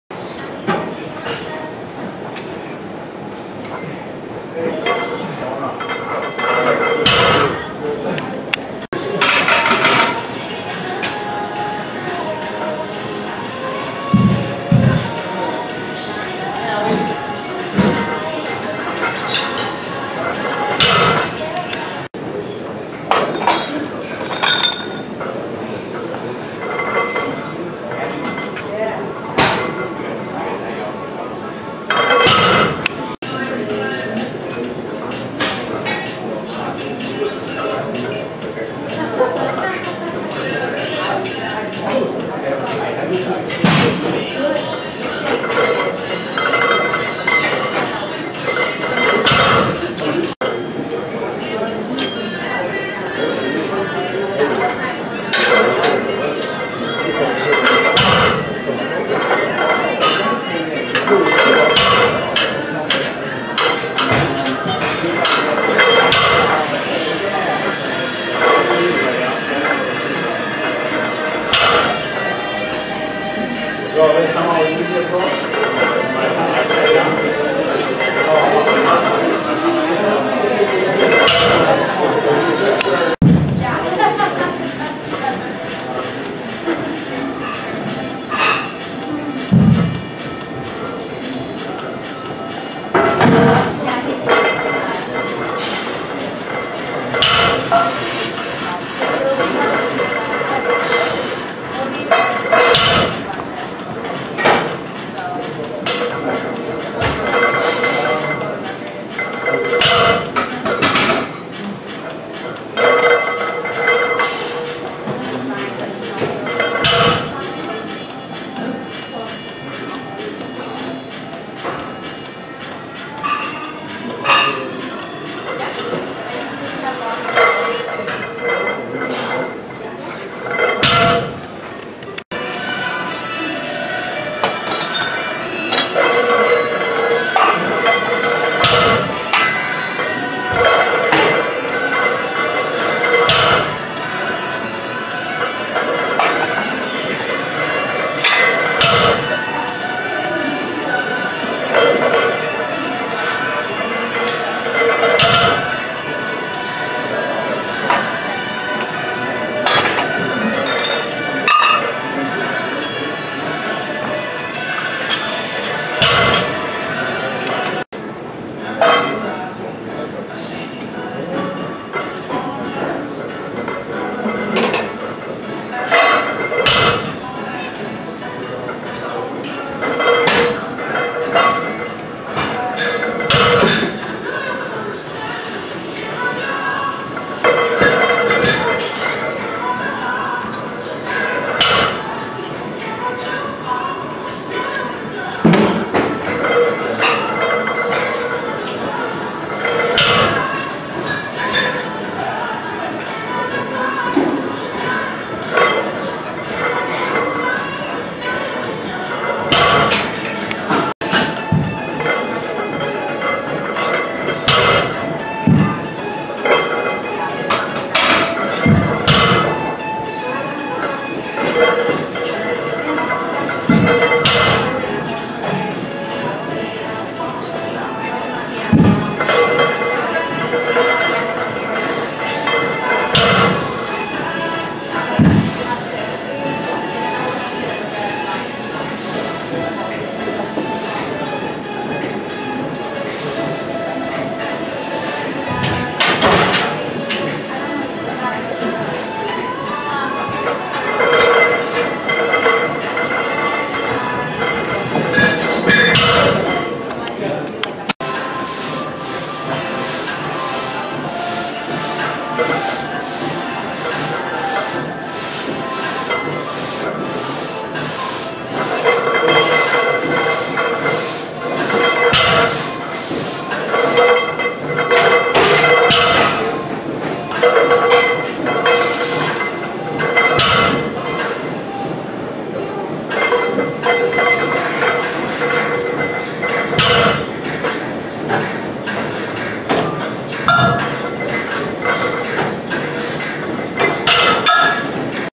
Deads for Reps
Hit 315x3x5
315 starting to feel like a piece of cake now :D